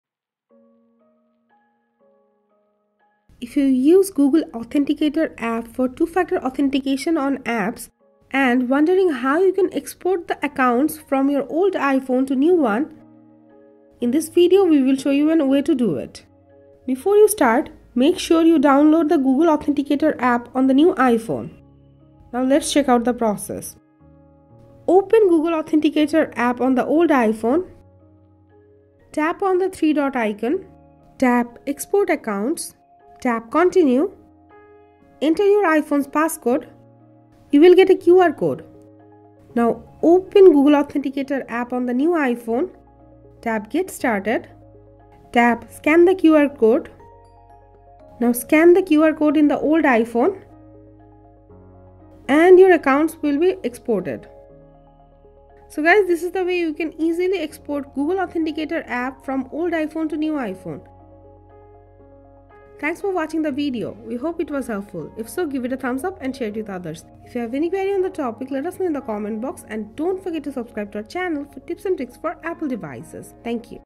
Transfer Google Authenticator to New iPhone (2021)
Looking for a way to move your Google Authenticator from old iPhone to new iPhone? In this video we will show the step by step process to transfer GoogleAuthenticator to new iPhone.

If you use Google Authenticator for 2-factor authentication and when you change iPhone, you need to transfer the accounts to new iPhone. You will find the process in video, check that out.